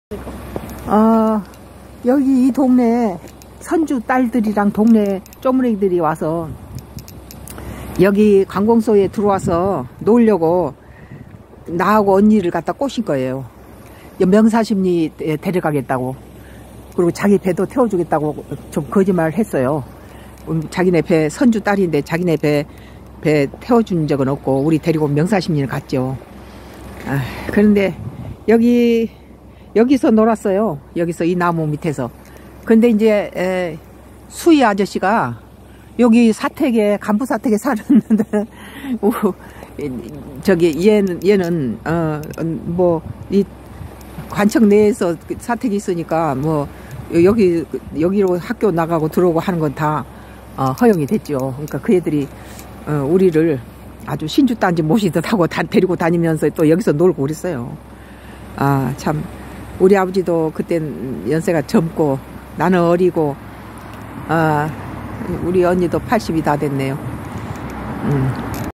어린시절 놀던 옛 나무가 그대로네@sungodcross
어린시절 67년전~잠깐 살았던 관공서 내부를 둘러보다~~!

아버지께서 사무관으로 부임하셔서 ~~!
잠깐 관공서내에 일본식 간부사택에서 살았는데~~!

오늘은 읍내 나온김에~~~언니랑 가위바위보 하며 아카시아잎을 떼어내며 놀던
관공서내의 간부사택으로 올라가던 계단에 올라가 보았다~~~!@sungodcross